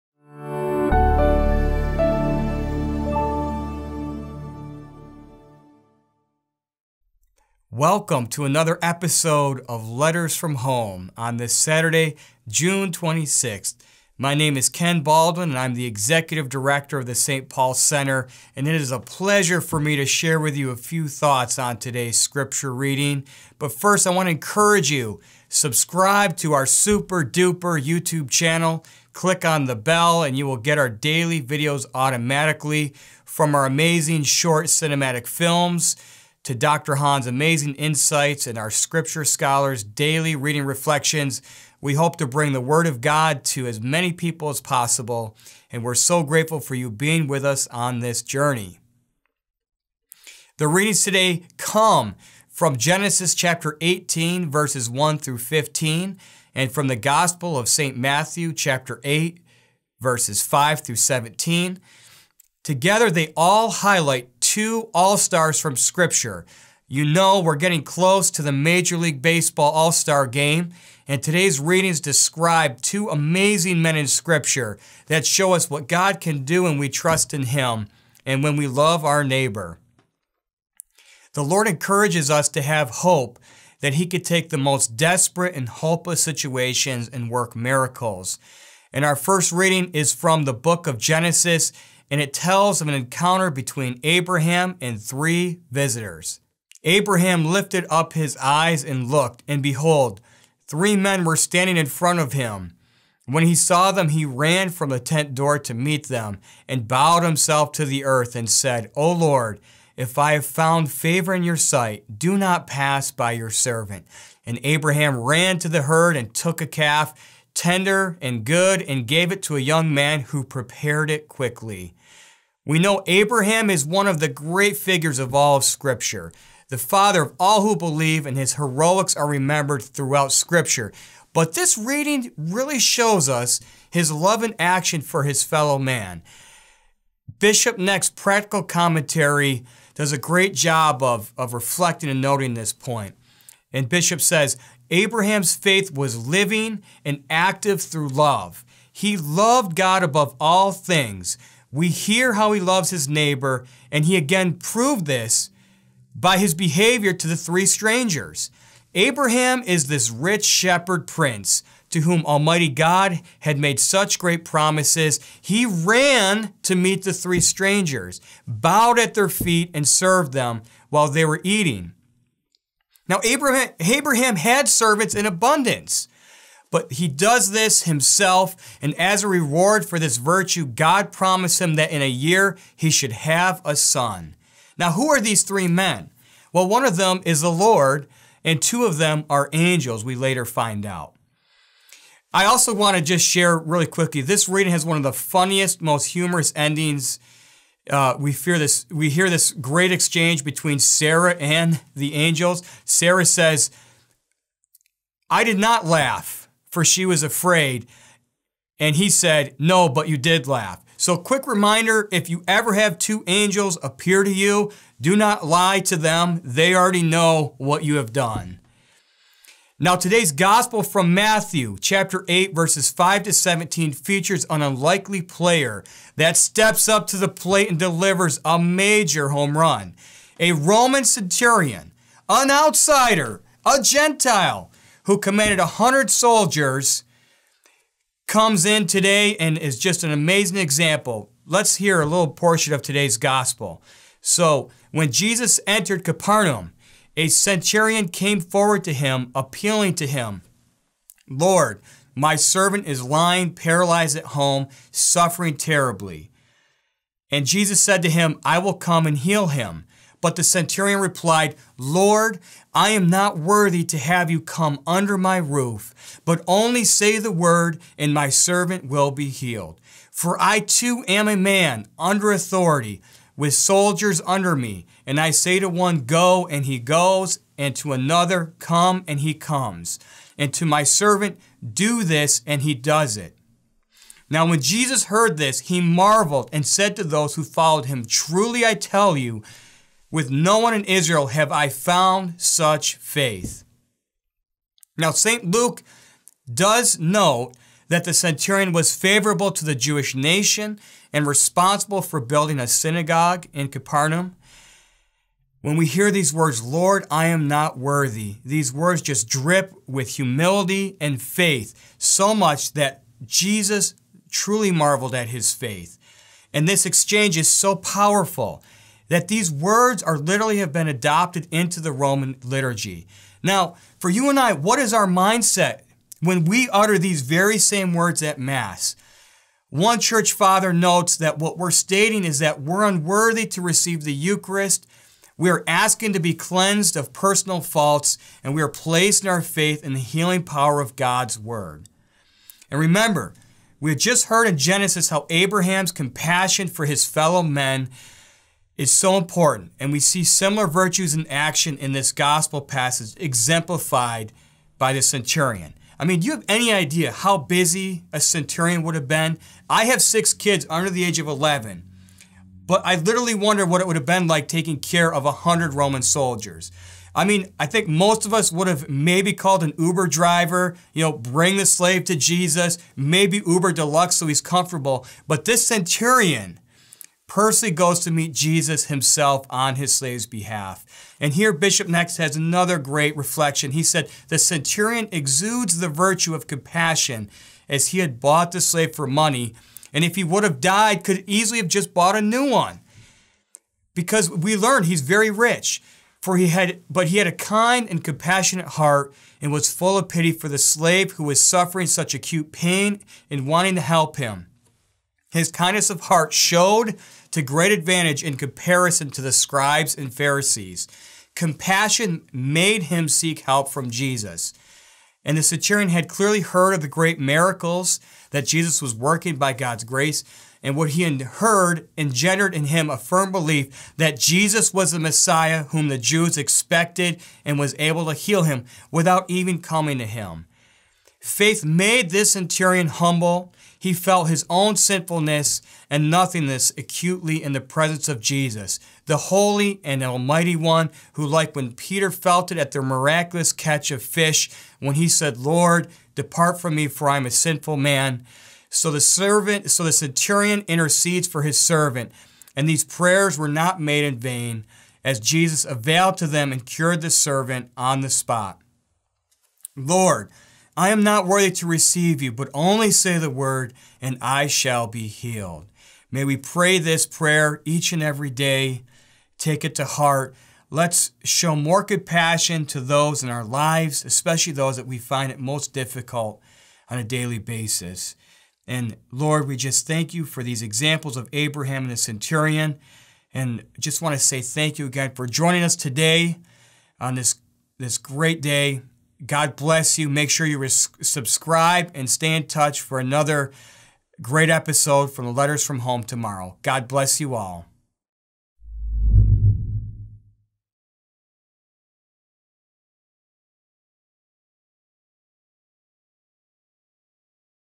Abraham and the Roman Centurion
Ken Baldwin, Executive Director of the St. Paul Center for Biblical Theology, gives a thoughtful reflection on the readings for mass on June 26, 2021.

Gn 18:1-15
Luke 1:46-47, 48-49, 50 and 53, 54-55
Mt 8:5-17

Learn more about Curtis Mitch and the St. Paul Center by visiting